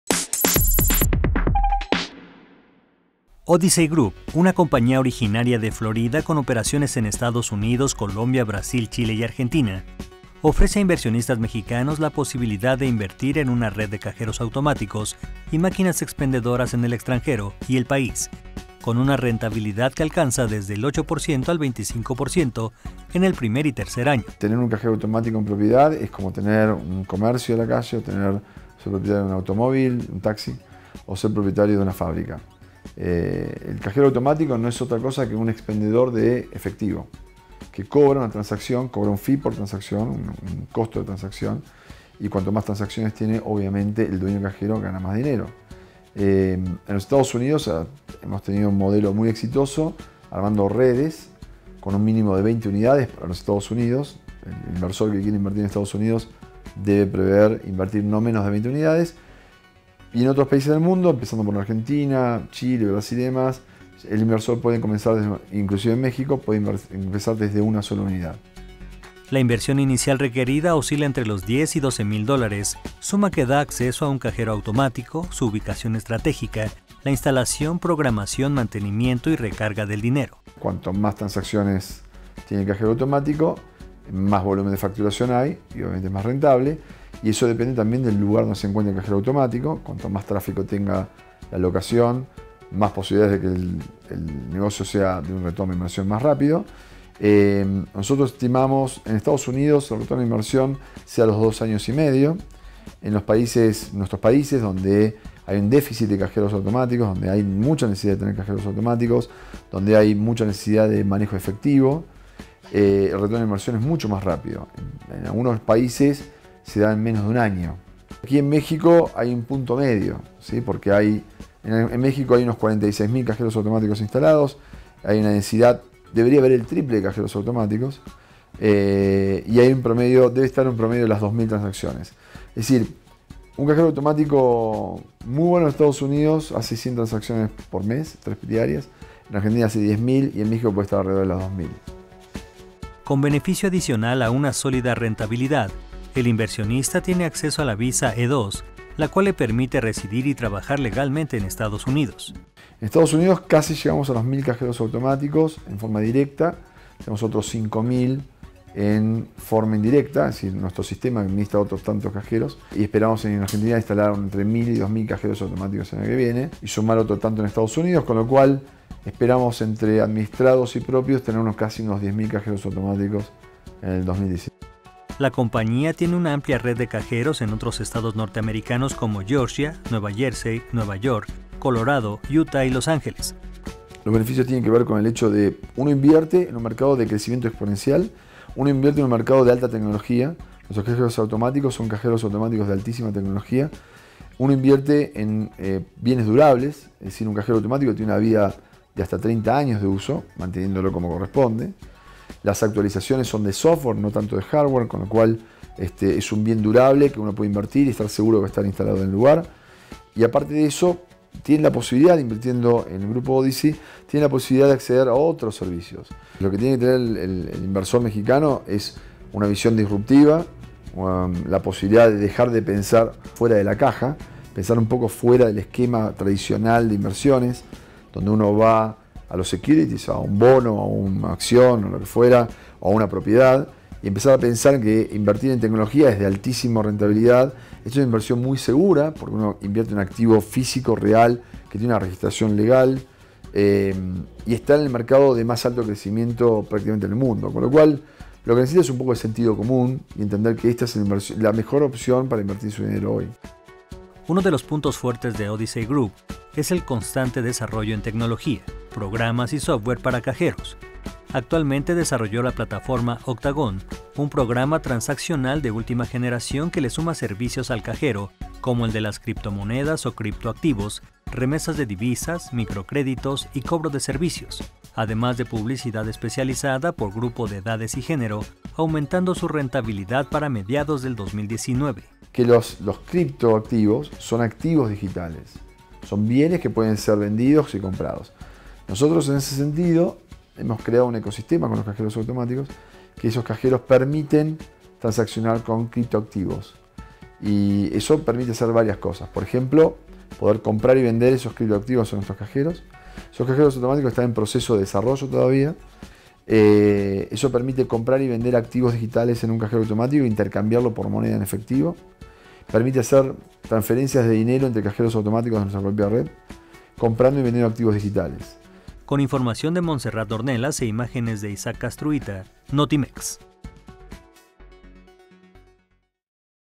Odyssey Group ofrece inversión en redes de cajeros automáticos en México y Estados Unidos
Odyssey Group una compañía originaria de Florida, con operaciones en Estados Unidos, Colombia, Brasil, Chile y Argentina, ofrece a inversionistas mexicanos la posibilidad de invertir en una red de cajeros automáticos y máquinas expendedoras en el extranjero y el país, con una rentabilidad que alcanza desde el 8% al 25% en el primer y tercer año.